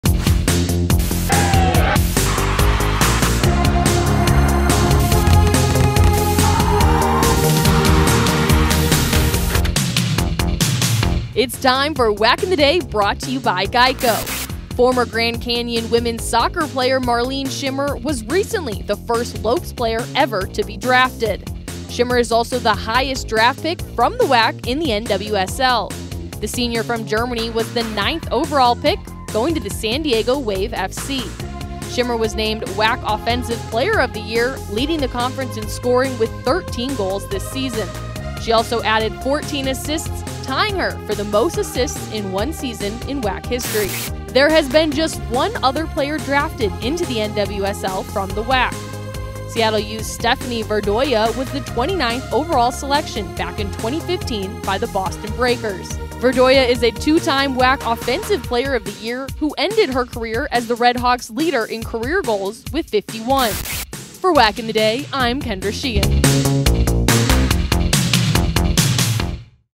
WAC in the Day - 1.7.21 - Marleen Schimmer Drafted
@GCU_WSoccer player Marleen Schimmer is the first ever @NWSL selection from the Lopes program!

In this episode we look back at the last time a WACwsoc player was selected in the draft.